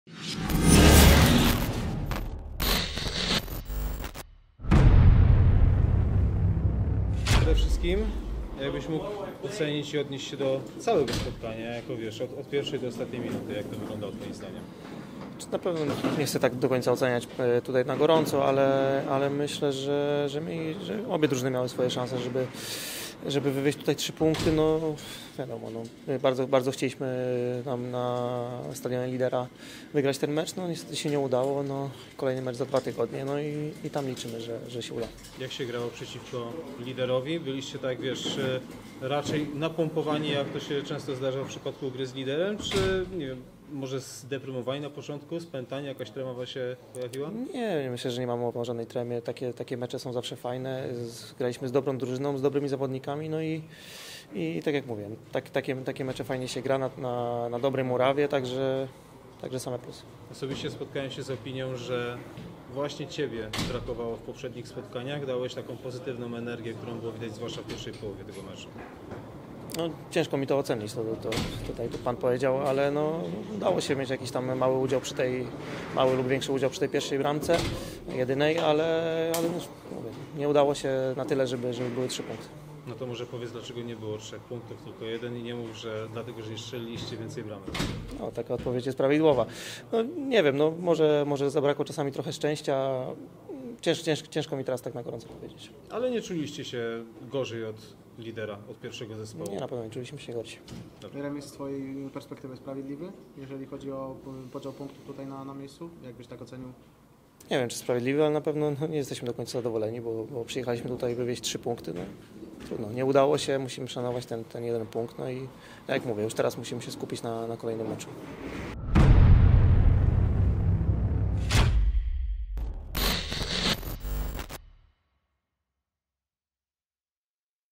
Jakub Łukowski po meczu z Miedzią Legnica
Wypowiedź Jakuba Łukowskiego po meczu 24. kolejki Fortuna 1 Ligi, Miedź Legnica - Korona Kielce 1:1.